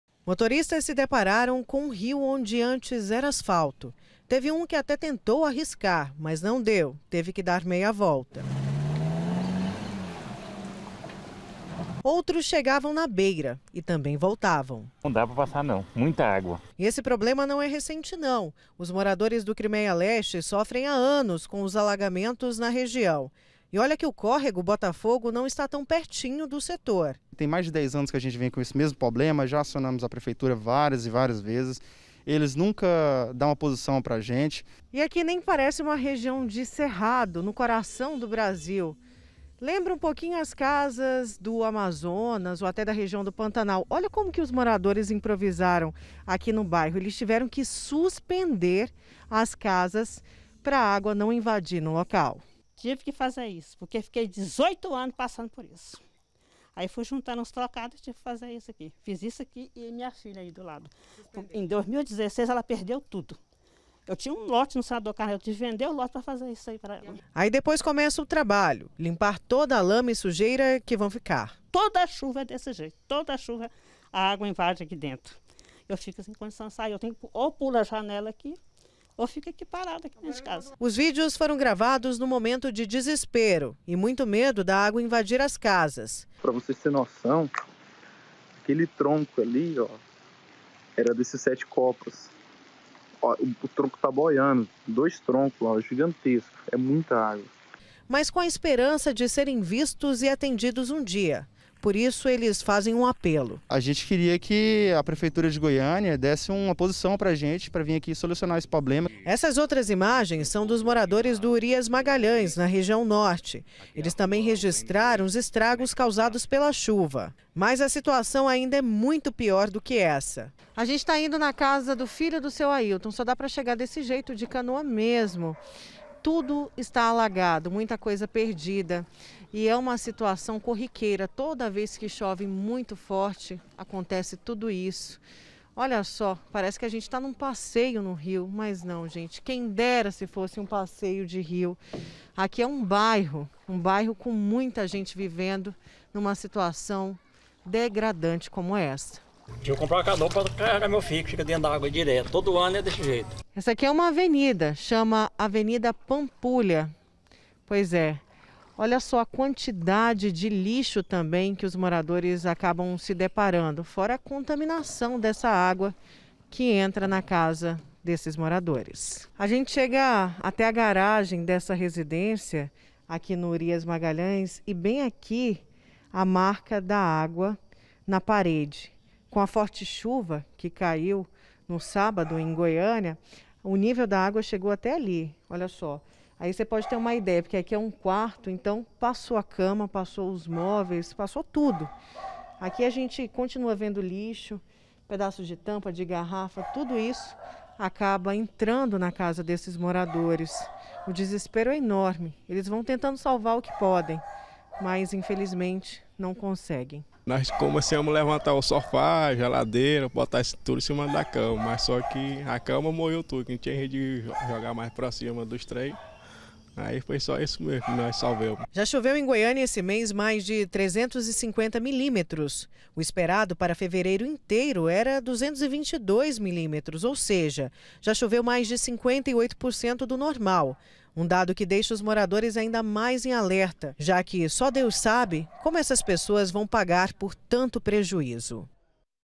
GOIÂNIA: CHUVA DEIXA RUAS ALAGADAS E MORADORES ILHADOS
NA REGIÃO NORTE, VÁRIAS RUAS FICARAM ALAGADAS E TEVE GENTE ATÉ USANDO CANOA PRA SAIR DE CASA.